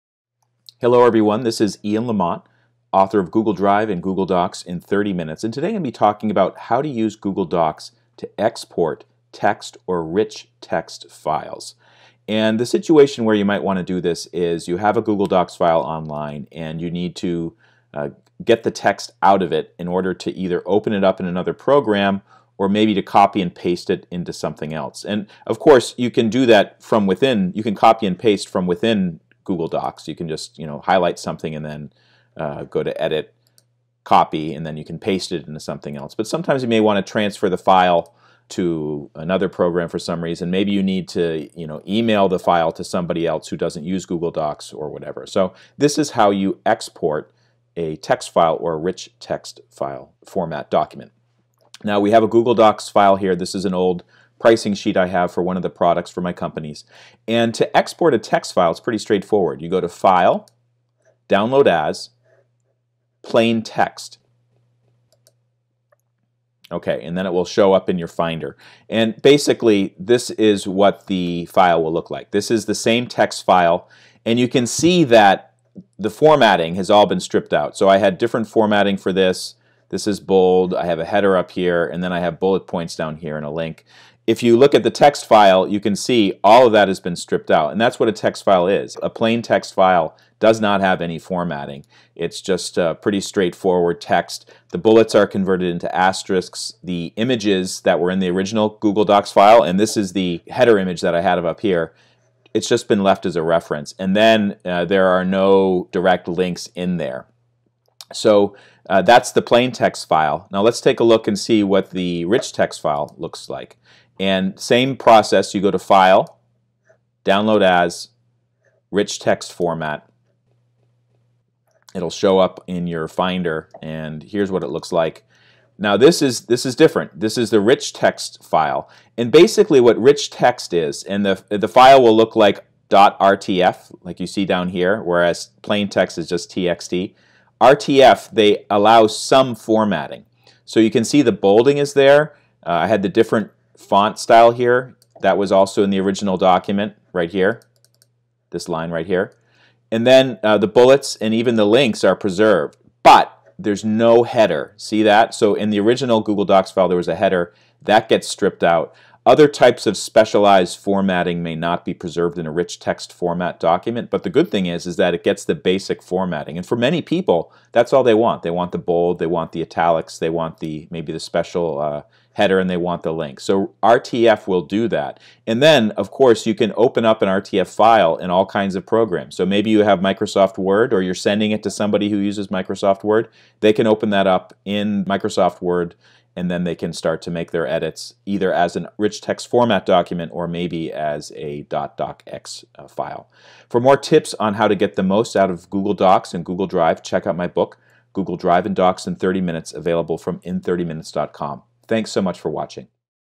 Export Google Docs as .txt or .rtf
If you use Google Docs, sometimes it is necessary to export the file as text or rich text. In this video, learn how to export to these two formats and why .rtf (rich text format) may be the better choice for some projects. Narrated by Ian Lamont, author of Google Drive & Docs In 30 Minutes.